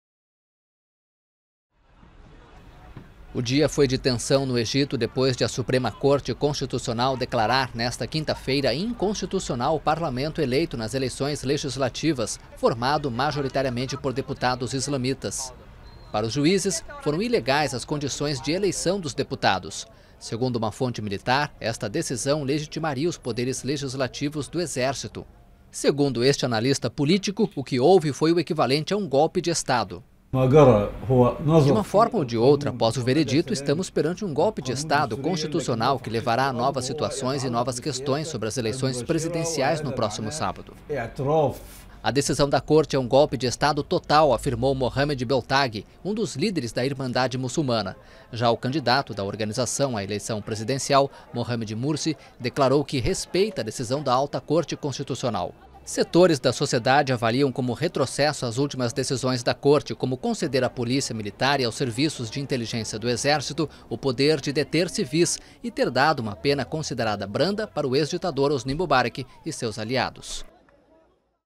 Tensão no Egito
A Suprema Corte Constitucional egípcia declarou nesta quinta-feira inconstitucional o Parlamento originado nas eleições legislativas, formado majoritariamente por deputados islamitas.